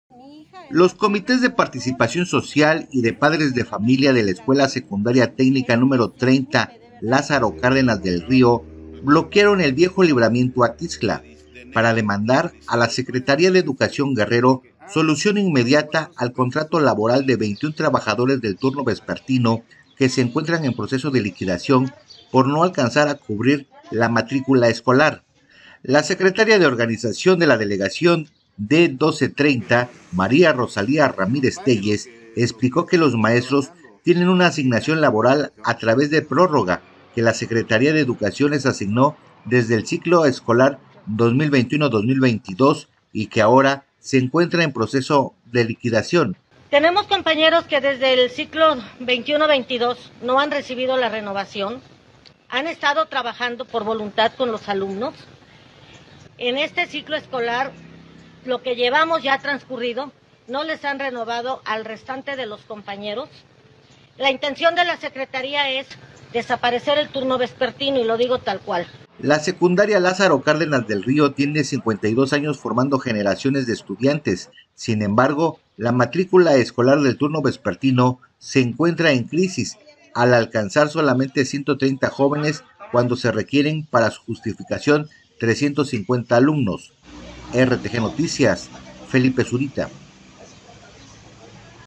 RTGnoticias | 21 docentes a punto de perder su trabajo por no alcanzar matrícula
Los comités de participación social y de padres de familia de la Escuela Secundaria Técnica número 30 Lázaro Cárdenas del Río, bloquearon el viejo libramiento a Tixtla para demandar a la Secretaría de Educación Guerrero solución inmediata al contrato laboral de 21 trabajadores del turno vespertino que se encuentran en proceso de liquidación por no alcanzar a cubrir la matrícula escolar.